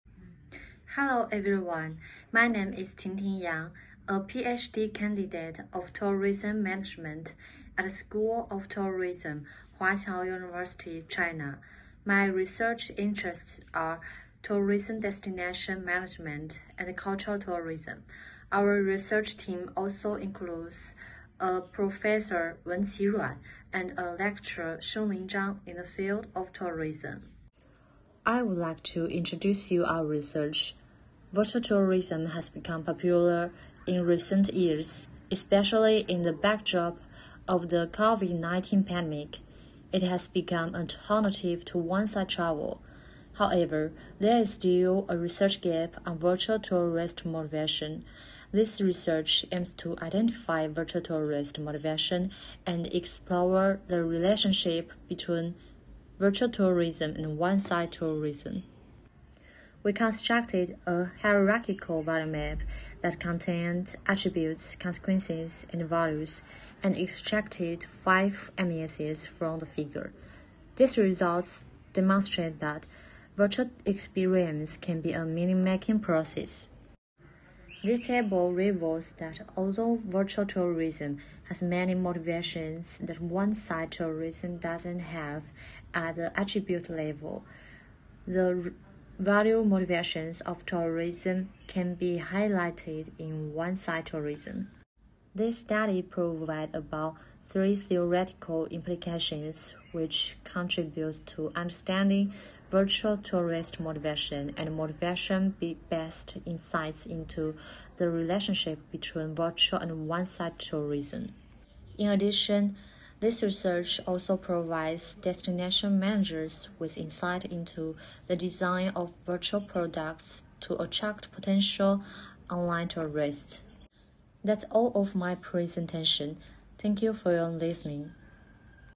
Virtual tourist motivation The differences between virtual tourism and on site tourism
Purpose
Virtual tourism has become popular in recent years. However, there is still a research gap on virtual tourist motivation. This study aims to identify virtual tourist motivation and explore the relationship between virtual tourism and on-site tourism.
Design/methodology/approach
This research identifies virtual tourist motivation based on Means-end chain (MEC) theory. Laddering interviews with 32 respondents were conducted to construct a hierarchical value map. Additionally, a motivation analysis of virtual tourism and on-site tourism was developed based on a review of the relevant literature.
Findings
This exploratory study revealed 12 attributes, 9 results and 4 values that virtual tourists wish to achieve and identified 5 means-end chains where self-satisfaction is the most important value-led motivation. Compared with on-site tourism motivations, virtual tourism shows possibilities of replacing, complementing and extending on-site tourism under certain circumstances. However, it significantly depends on whether tourists are attracted by the technical characteristics, security and experience conditions of virtual tourism.
Originality/value
This study contributes to understanding virtual tourist motivation and offers motivation-based insights into the relationship between virtual and on-site tourism. Managerial implications on how to attract potential online tourists are also provided.